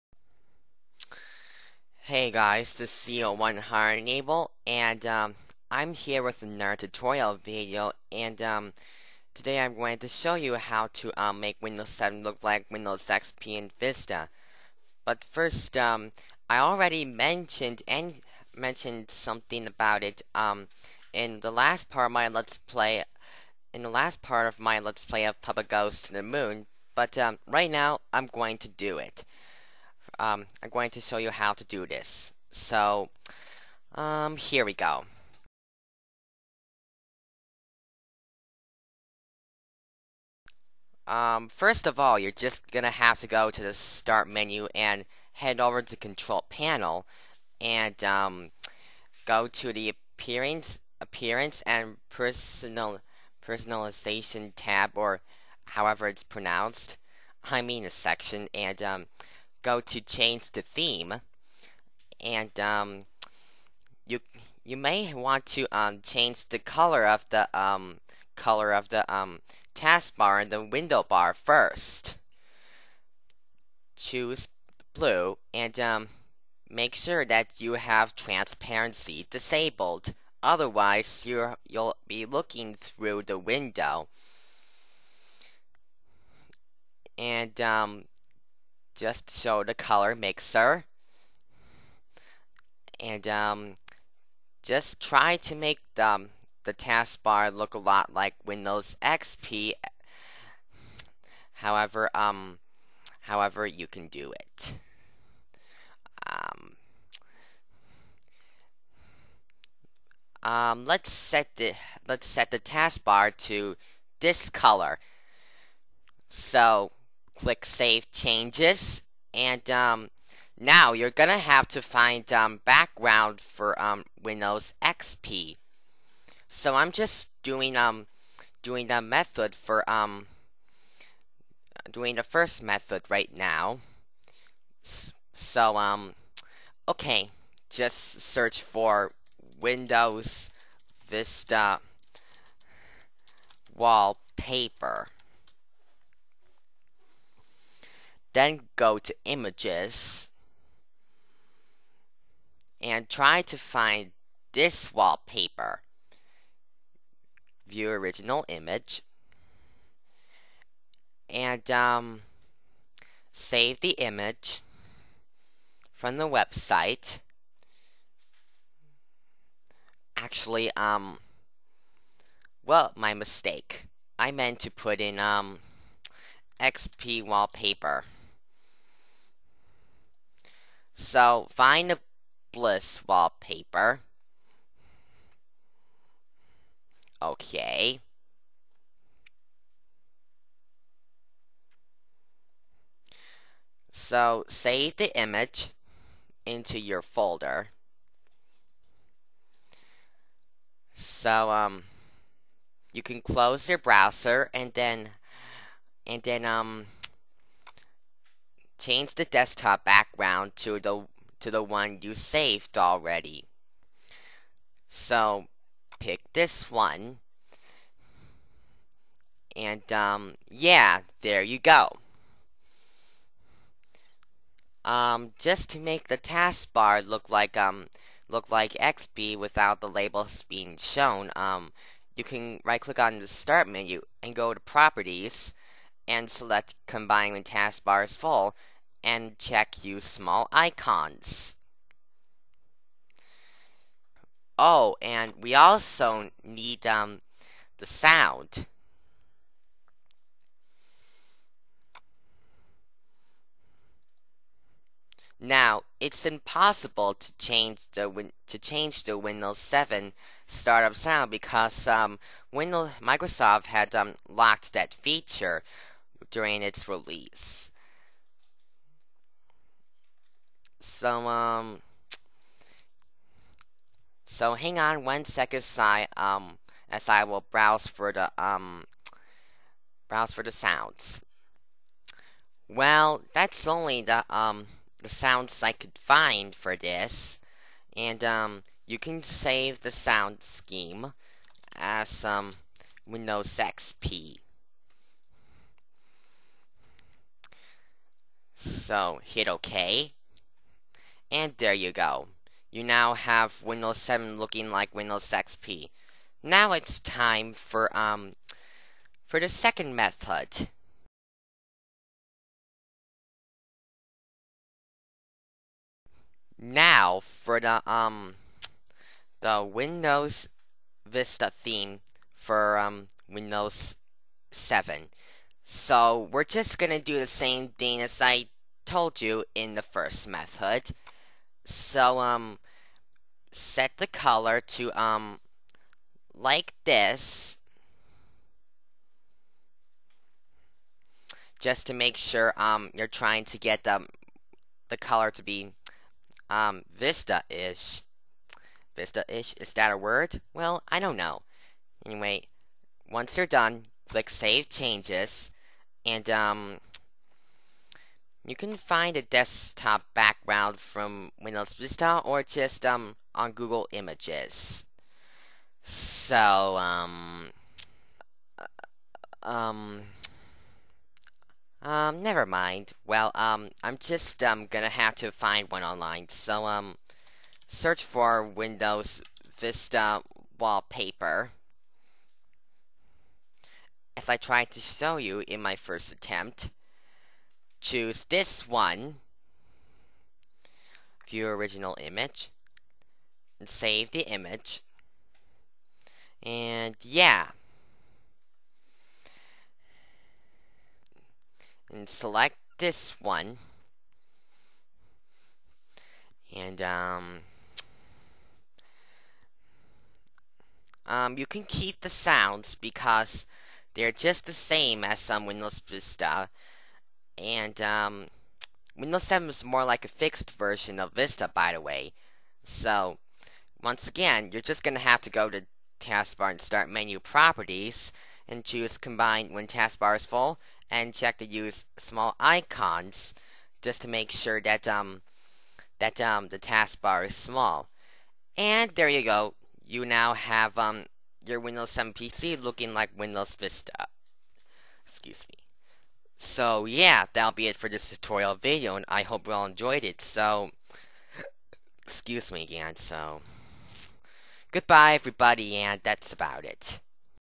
How To Make Windows 7 Look Like Windows XP and Vista
This tutorial video will show you how to make your Windows 7 PC look like XP and Vista. It's kind of like my other tutorial on how to make Windows XP look like Vista, though. Enjoy!